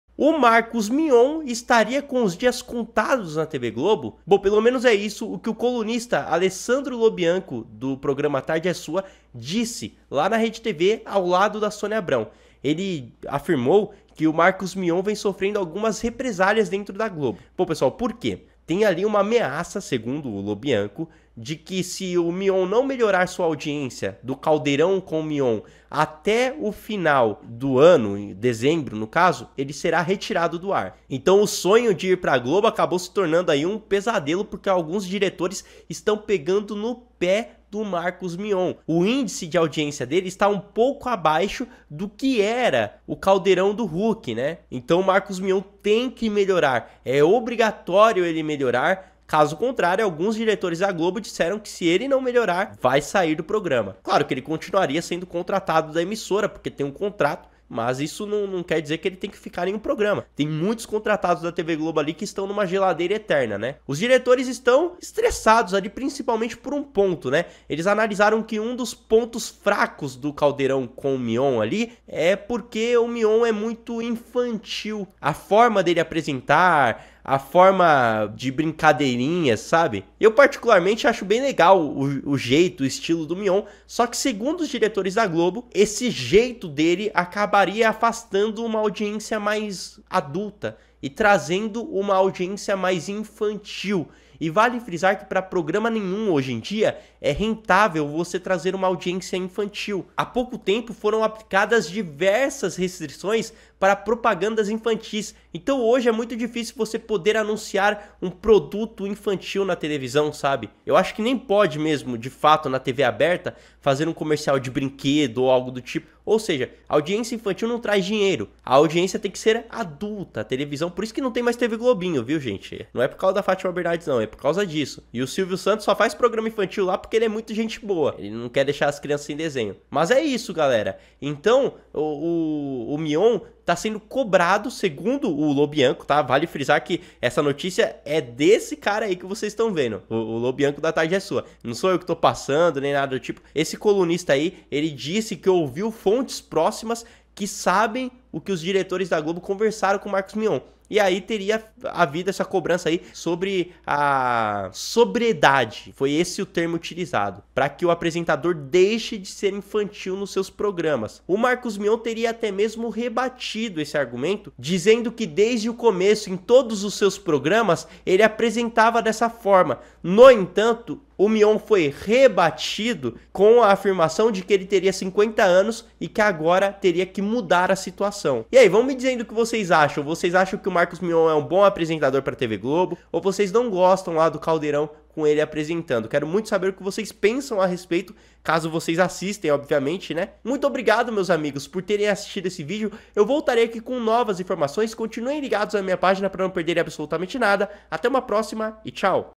🔴FIM DO SONHO: GLOBO AMEAÇA MARCOS MION, QUE SE NÃO OBEDECER, SAIRÁ DO AR!
Marcos Mion é substituto do Luciano Huck com o programa 'Caldeirão' na TV Globo.